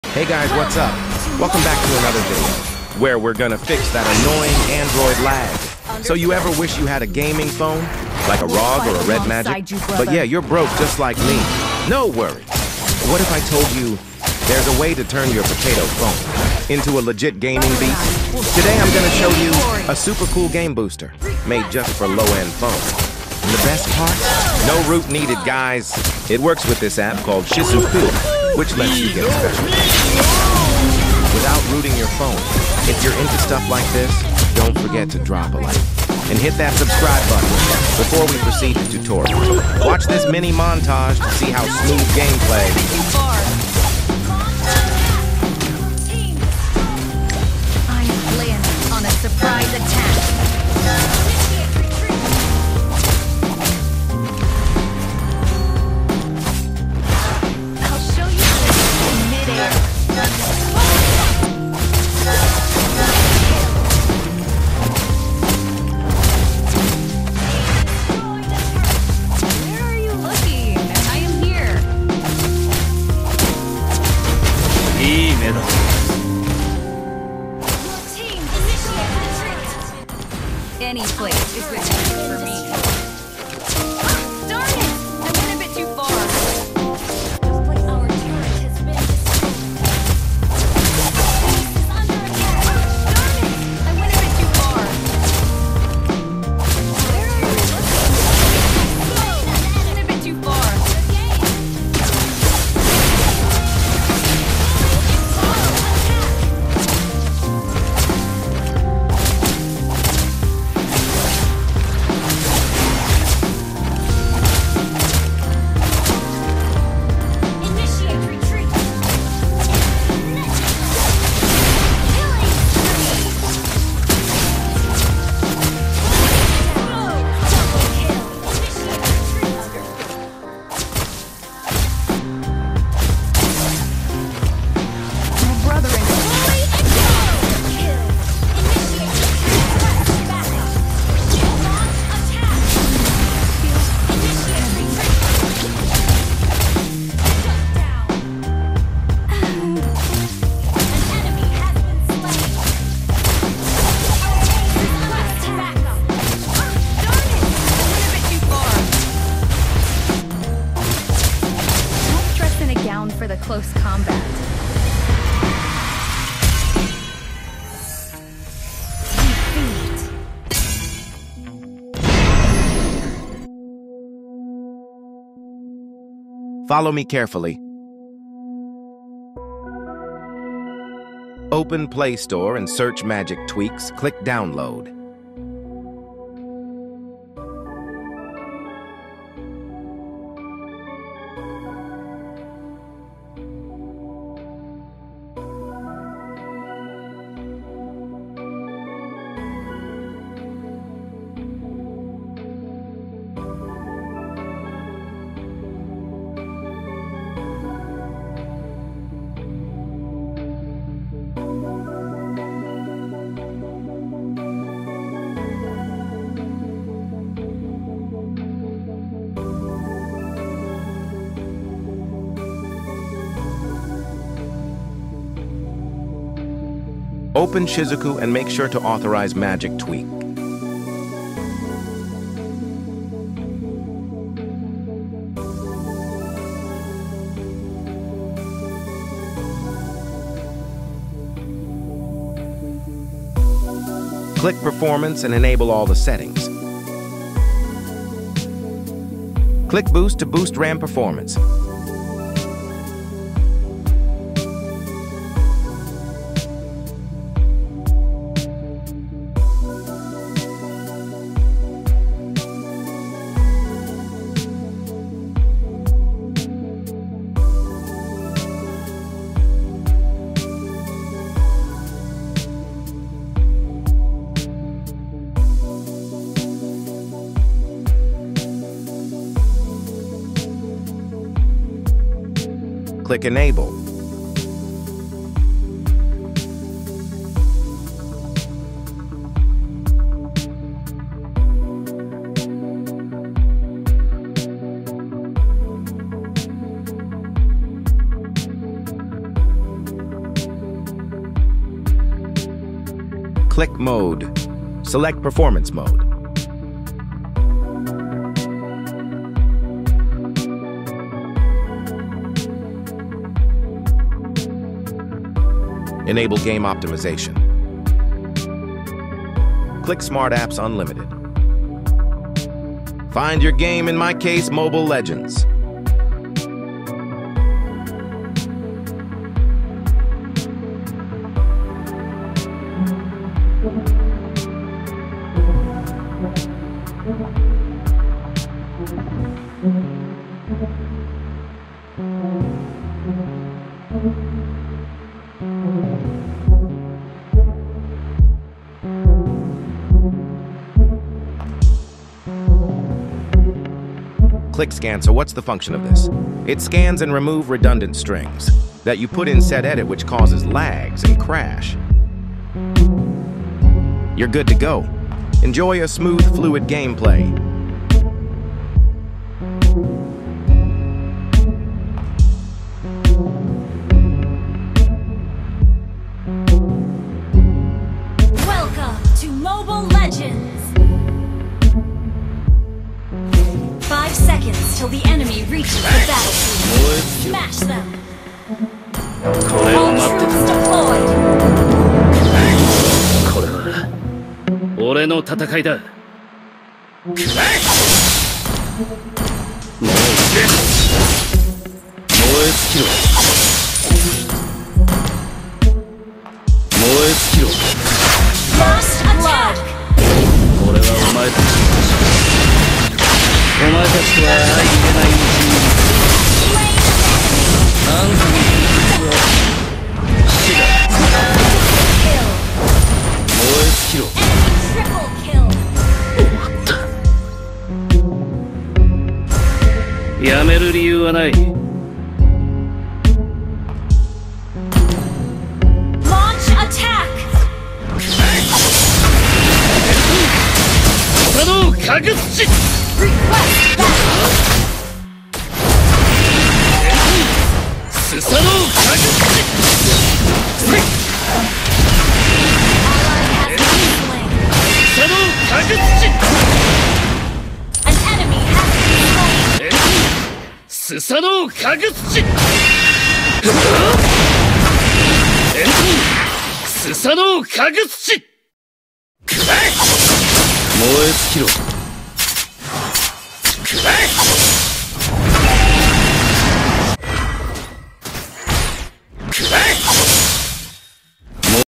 Shizuku Gamebooster: Turn Low-end RAM phone into a High Ram Gaming Phone! Fix FPS Drop in Android 🔥
Description:
Tired of lag ruining your Mobile Legends or Call of Duty Mobile matches? 😩 In this video, I’ll show you how to turn your low-end Android into a high-RAM gaming machine using Shizuku Gamebooster — no root required! 🚀 Say goodbye to frame drops and hello to smooth, boosted gameplay in 2025.

🎯 Topics Covered:
✅ Boost RAM on low-end Android phones
✅ Fix FPS drops and lag spikes
✅ Best Shizuku Gamebooster settings for 2025
✅ Smooth gameplay on MLBB, CODM, and more
✅ No root, no bloat, 100% safe to use

💬 What device are you using? Drop it in the comments! 🔥📱🎮